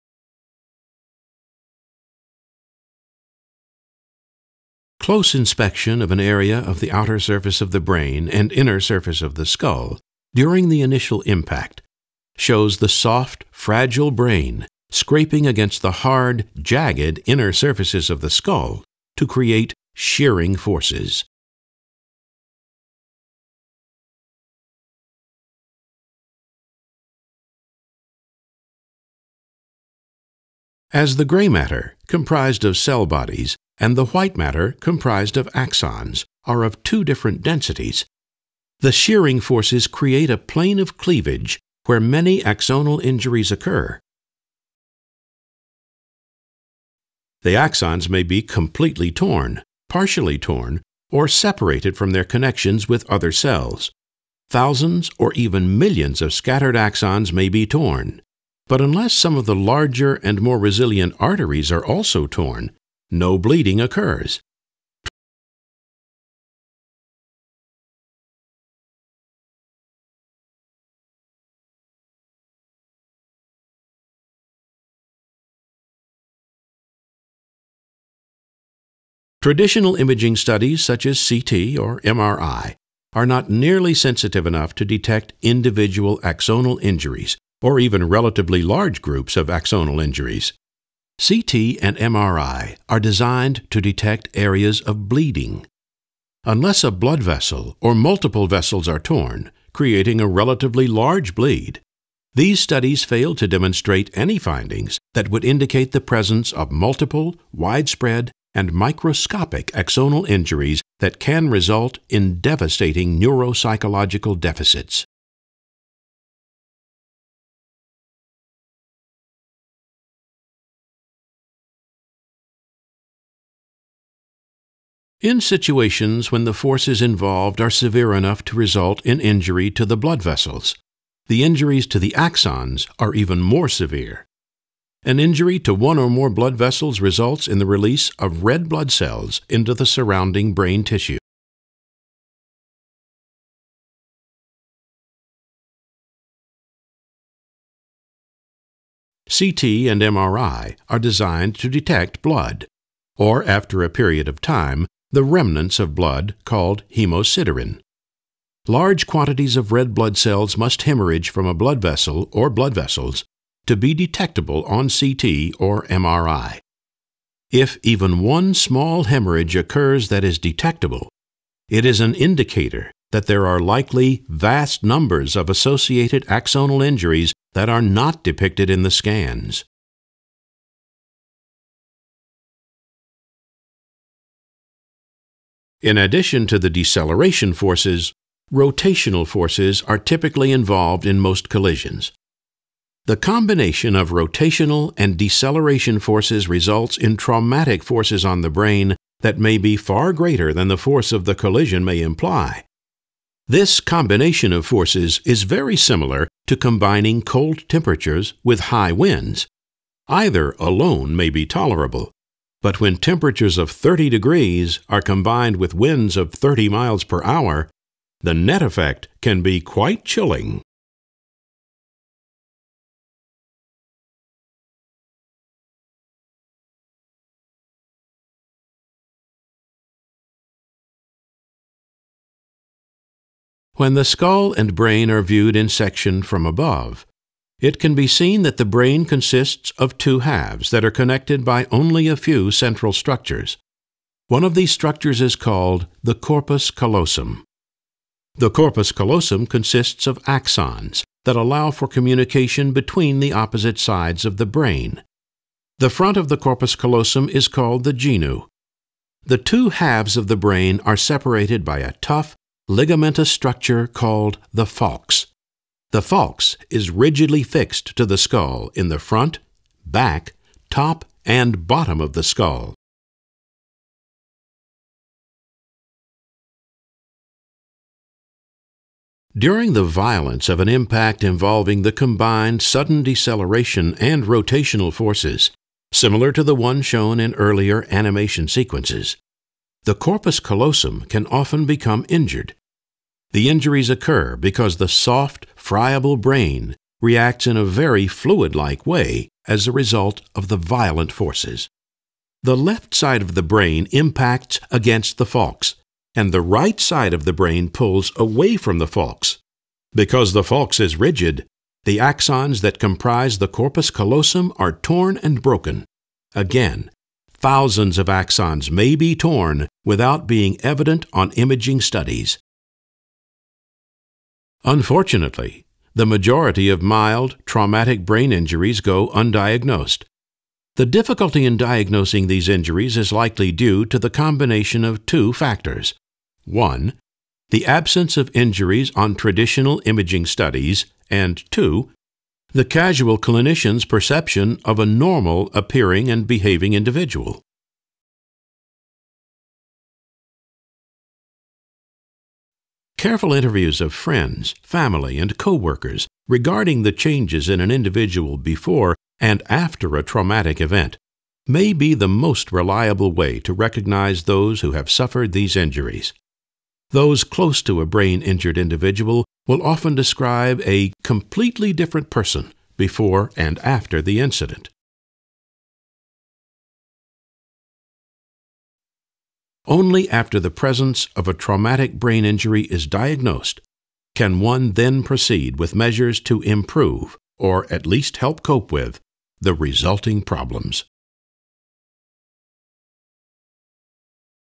Traumatic Brain Injury (TBI) Part 2: 2D/3D Medical Animation
- Traumatic Brain Injury (TBI) - In correlation with some of the nation's leading medical experts and trial attorneys who specialize in "mild traumatic brain injury (mild TBI)," MediVisuals has developed an interactive animated software that demonstrates how traumatic brain injuries can occur without a significant blow to the head and without being evidenced on imaging studies.

Animated sequences based upon crash tests demonstrate the movements associated with a "typical" sudden deceleration incident. Multiple other animation sequences explain the involved anatomy and show the soft, fragile brain impacting on the hard and uneven inner surface of the skull. The sequences demonstrate how multiple axonal injuries can occur from the shearing forces (both with and without hemorrhage and both with and without positive findings on imaging studies). An additional animation sequence demonstrates how the combined deceleration and rotational forces result in axonal injury in the corpus callosum.